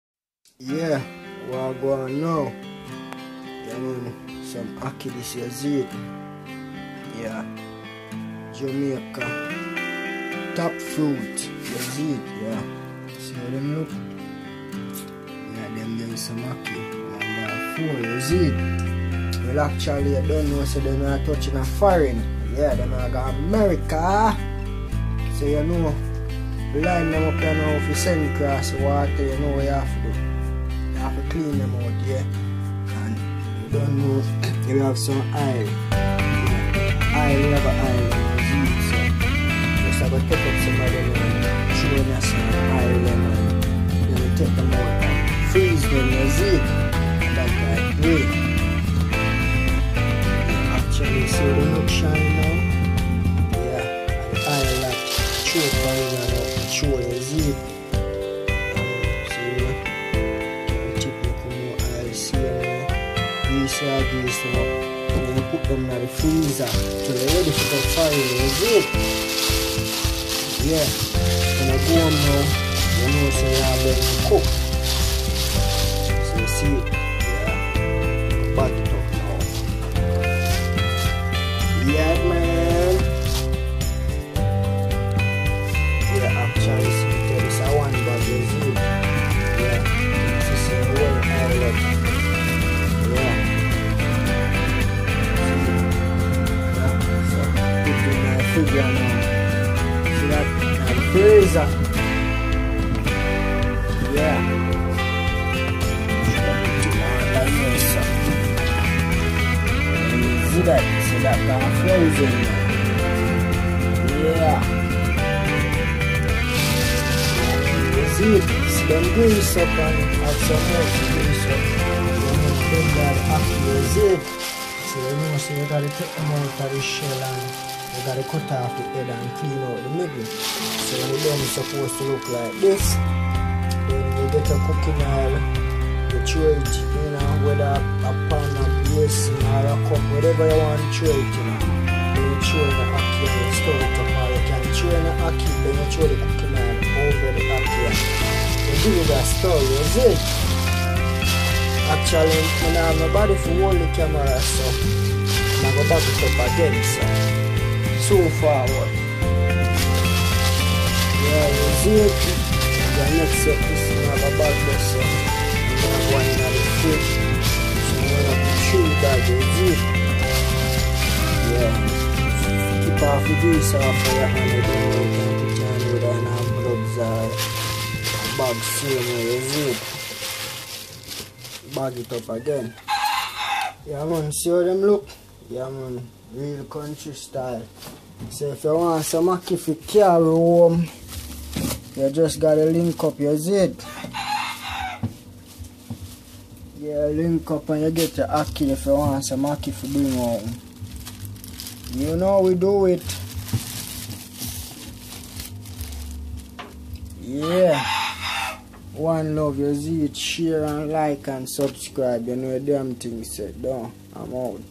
This is what you do to bring ackee in the United State
Yea man that is Jamaica ackee if you are leaving Jamaica and want some ackee you can link me OK any Jamaican fruits you want give me a call